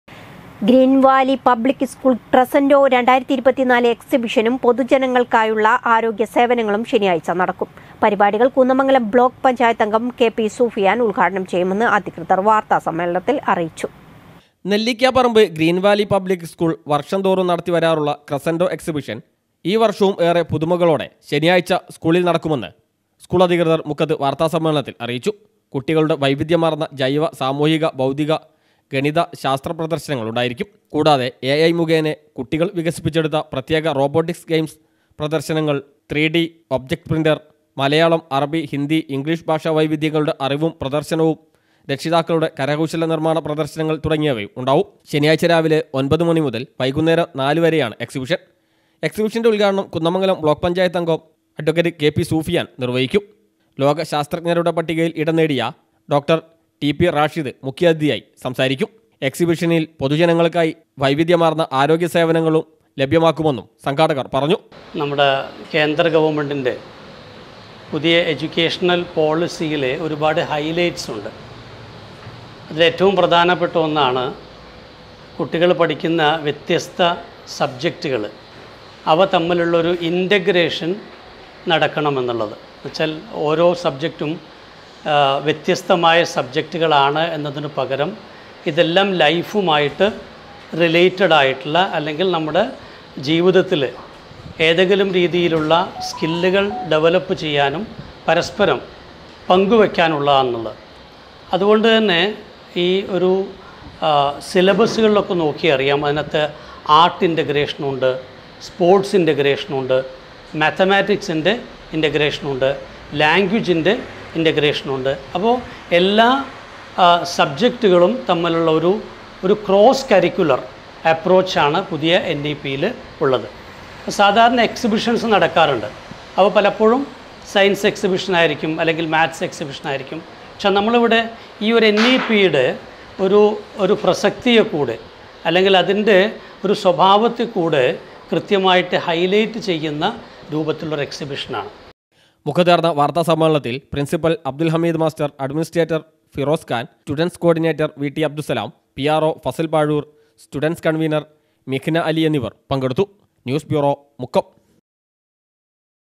ഗ്രീൻ വാലി പബ്ലിക് സ്കൂൾ ക്രസൻ്റോ 2024 എക്സിബിഷനും പൊതുജനങ്ങൾക്കായുള്ള ആരോഗ്യ സേവനങ്ങളും ശനിയാഴ്ച
ഗ്രീൻ വാലി പബ്ലിക് സ്കൂൾ ക്രസൻ്റോ 2024 എക്സിബിഷനും പൊതുജനങ്ങൾക്കായുള്ള ആരോഗ്യ സേവനങ്ങളും ശനിയാഴ്ച നടക്കും.